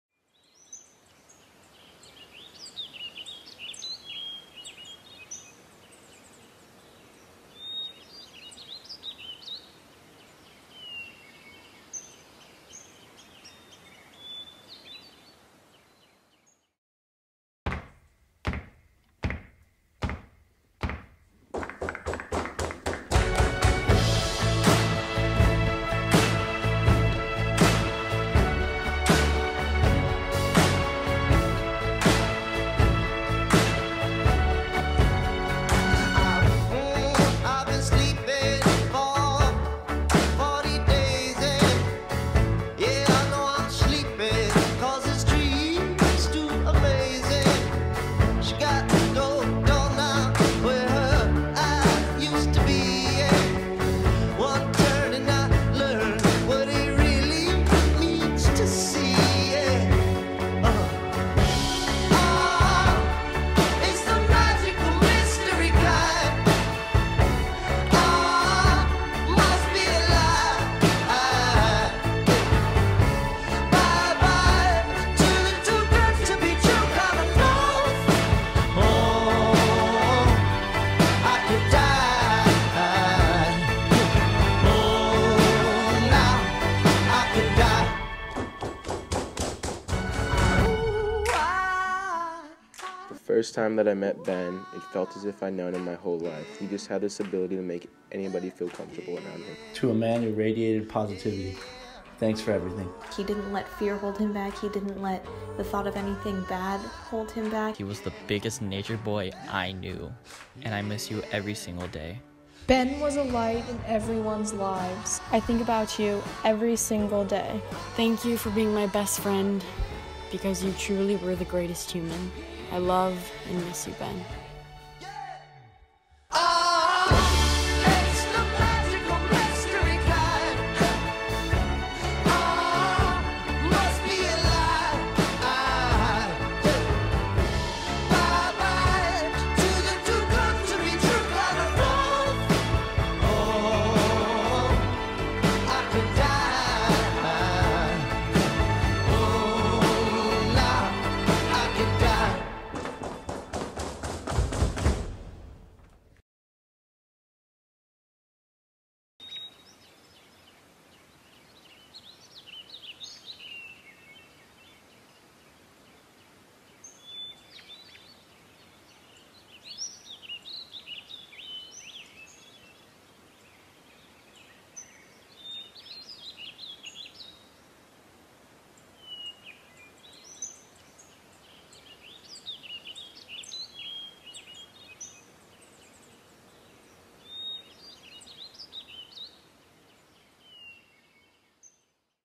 BEN - Short Film
Ben, everyones best friend. You were truly one of a kind, and no one will ever forget everything you brought into this world. We all love you, and we all miss you Ben.